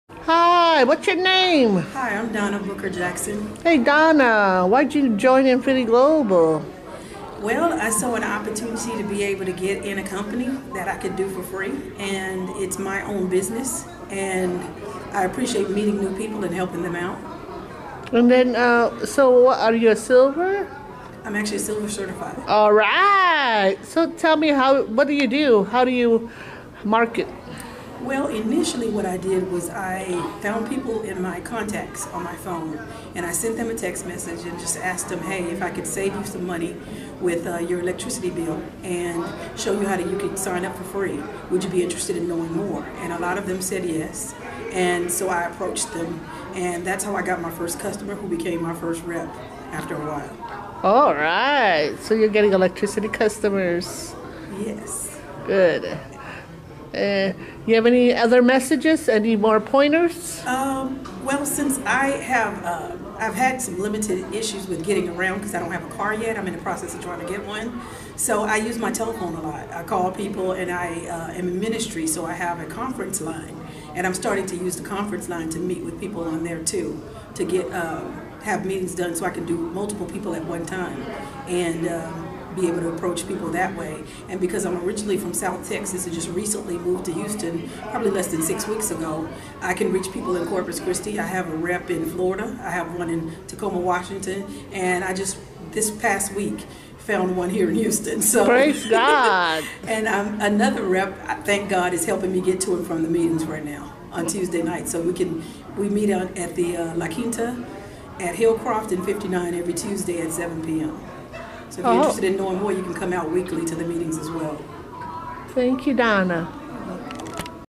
Donna Joined Enfiniti Global for the Free Opportunity to make Money.
Donna Joined Enfiniti Global for the Free Opportunity to make Money.Donna shares her secret to her success. This is my Business in making money . Everyone uses and got to pay monthly and I am so happy I got into this company i can share.